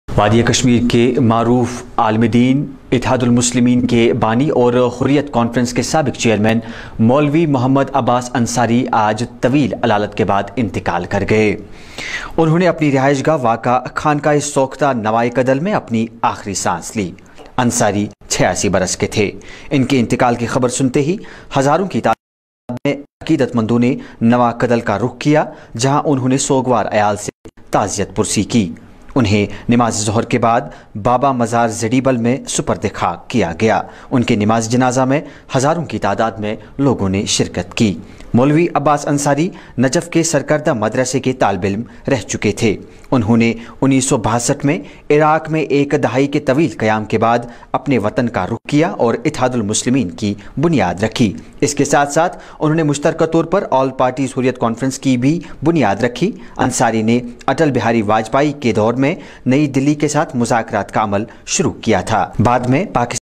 Founder and chairman of Ittihadul Musilmeen, Mohammad Abbas Ansari passed away
Founder and chairman of Ittihadul Musilmeen, Mohammad Abbas Ansari passed away after a prolonged illness at his residence in Khankahi sokhta  Nawakadal in Srinagar.